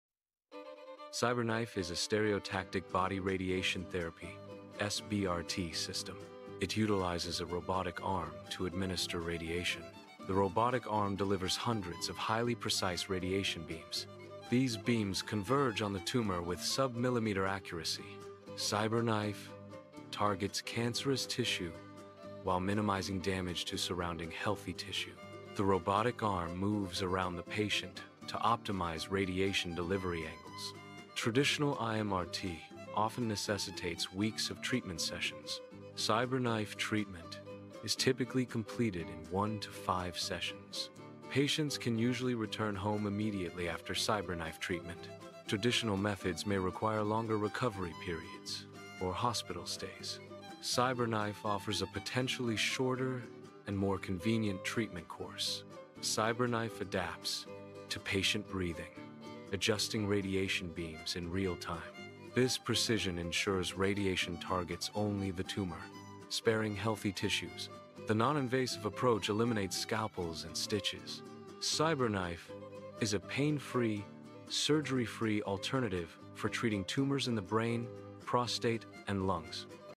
CyberKnife in 1 Minute | What Is CyberKnife? How CyberKnife Destroys Tumors Without Surgery
CyberKnife is revolutionizing cancer treatment — using robotic precision to destroy tumors without surgery.
In this short video from Nourish Today, we explain what CyberKnife is, how it works, and why patients worldwide are choosing CyberKnife SBRT for prostate, lung, brain, and spine tumors.

CyberKnife is a form of Stereotactic Body Radiation Therapy (SBRT) that delivers hundreds of pinpoint radiation beams from multiple angles, minimizing damage to healthy tissues. Unlike traditional IMRT, it requires no incision and fewer treatment sessions.

👉 Watch till the end to learn how robotic precision meets radiation power in modern cancer care.

📚 References:

Mayo Clinic – CyberKnife Overview

Cleveland Clinic – SBRT Explained

WebMD – CyberKnife Radiotherapy

Health line – Cancer Radiation Options

Harvard Health Publishing